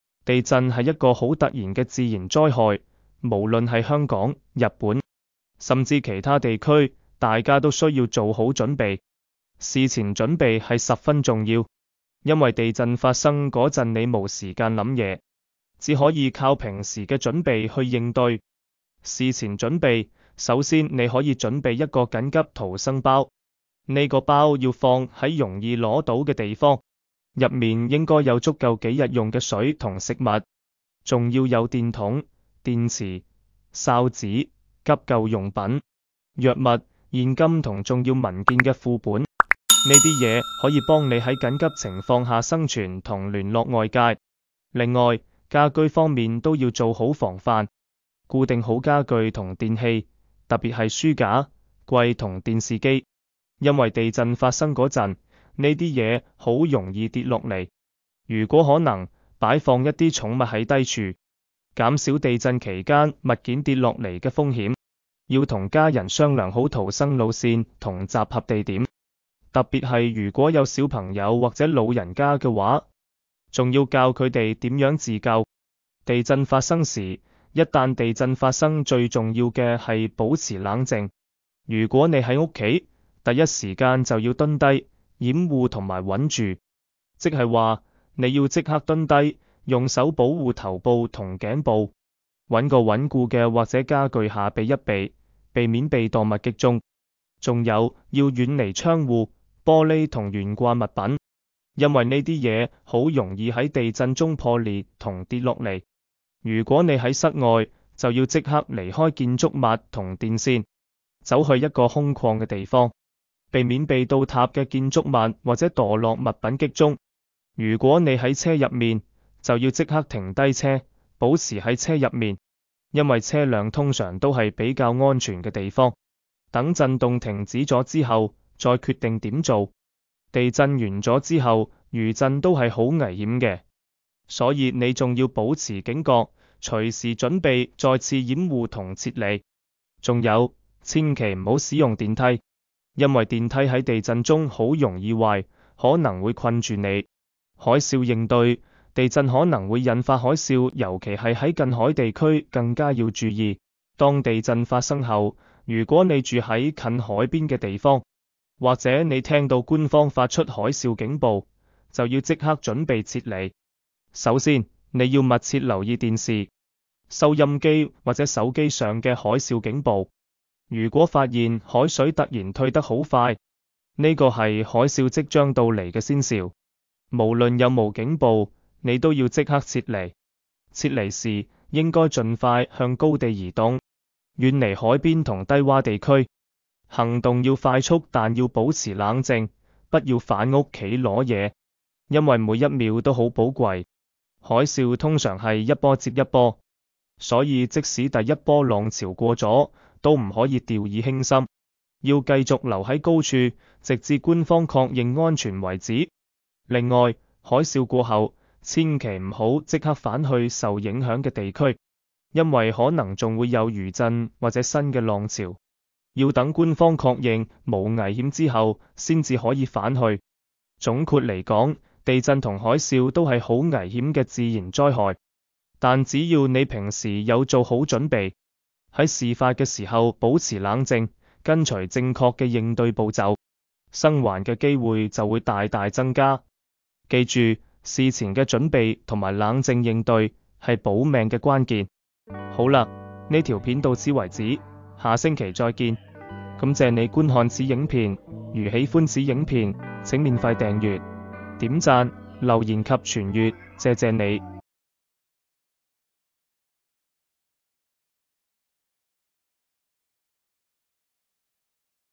地震, 海嘯 Stephen Lifestyle 生活品味
地震係一個好突然嘅自然災害，無論係香港、日本，甚至其他地區，大家都需要做好準備。事前準備係十分重要，因為地震發生嗰陣你無時間諗嘢，只可以靠平時嘅準備去應對。

事前準備：
首先，你可以準備一個緊急逃生包，呢個包要放喺容易攞到嘅地方。入面應該有足夠幾日用嘅水同食物，仲要有電筒、電池、哨子、急救用品、藥物、現金同重要文件嘅副本。呢啲嘢可以幫你喺緊急情況下生存同聯絡外界。

另外，家居方面都要做好防範。固定好家具同電器，特別係書架、櫃同電視機，因為地震發生嗰陣，呢啲嘢好容易跌落嚟。如果可能，擺放一啲重物喺低處，減少地震期間物件跌落嚟嘅風險。要同家人商量好逃生路線同集合地點，特別係如果有小朋友或者老人家嘅話，仲要教佢哋點樣自救。

地震發生時：
一旦地震發生，最重要嘅係保持冷靜。如果你喺屋企，第一時間就要蹲低、掩護同埋穩住。即係話，你要即刻蹲低，用手保護頭部同頸部，搵個穩固嘅枱或者家具下避一避，避免被墮物擊中。仲有，要遠離窗戶、玻璃同懸掛物品，因為呢啲嘢好容易喺地震中破裂同跌落嚟。

如果你喺室外，就要即刻離開建築物同電線，走去一個空曠嘅地方，避免被倒塌嘅建築物或者墮落物品擊中。如果你喺車入面，就要即刻停低車，保持喺車入面，因為車輛通常都係比較安全嘅地方。等震動停止咗之後，再決定點做。

地震完咗之後，餘震都係好危險嘅，所以你仲要保持警覺，隨時準備再次掩護同撤離。仲有，千祈唔好使用電梯，因為電梯喺地震中好容易壞，可能會困住你。

海嘯應對：
地震可能會引發海嘯，尤其係喺近海地區更加要注意。當地震發生後，如果你住喺近海邊嘅地方，或者你聽到官方發出海嘯警報，就要即刻準備撤離。

首先，你要密切留意電視、收音機或者手機上嘅海嘯警報。如果發現海水突然退得好快，呢個係海嘯即將到嚟嘅先兆，無論有無警報，你都要即刻撤離。

撤離時，應該盡快向高地移動，遠離海邊同低窪地區。行動要快速但要保持冷靜，不要返屋企攞嘢，因為每一秒都好寶貴。海嘯通常係一波接一波，所以即使第一波浪潮過咗，都唔可以掉以輕心，要繼續留喺高處，直至官方確認安全為止。

另外，海嘯過後，千祈唔好即刻返去受影響嘅地區，因為可能仲會有餘震或者新嘅浪潮。要等官方確認無危險之後，先至可以返去。

總括嚟講，地震同海嘯都係好危險嘅自然災害，但只要你平時有做好準備，喺事發嘅時候保持冷靜，跟隨正確嘅應對步驟，生還嘅機會就會大大增加。記住，事前嘅準備同埋冷靜應對，係保命嘅關鍵。

好嘞，呢條片到此為止，下星期再見。